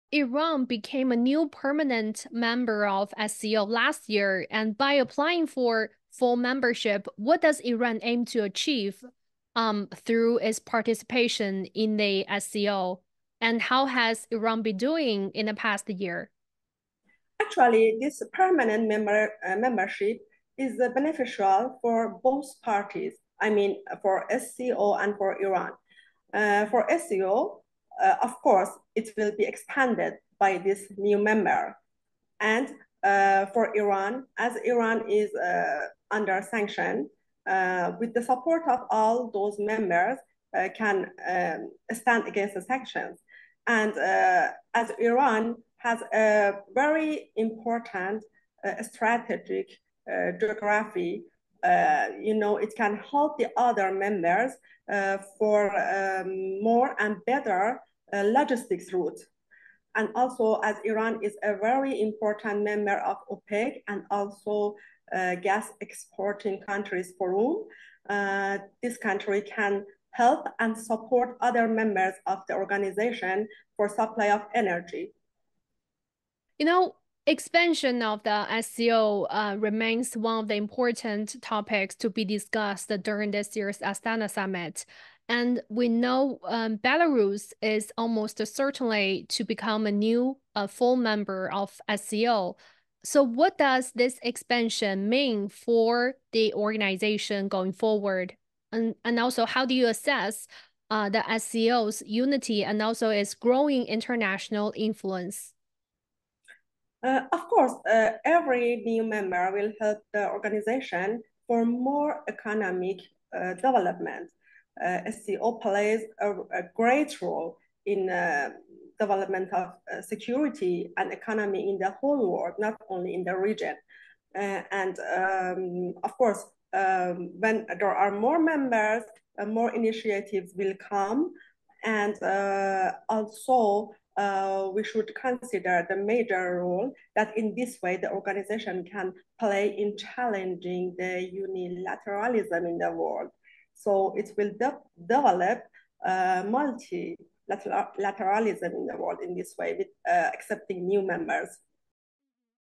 Mahnaz Abdi: Iran's energy supply could boost economy and trade within the SCO family
This year marks Iran's first year as an official member of the SCO. What role does Iran play in the SCO family? I spoke with Tehran Times reporter Mahnaz Abdi. She noted that Iran's participation could support the energy supply for members countries, and boosting economic, trade, and logistics development for the SCO family.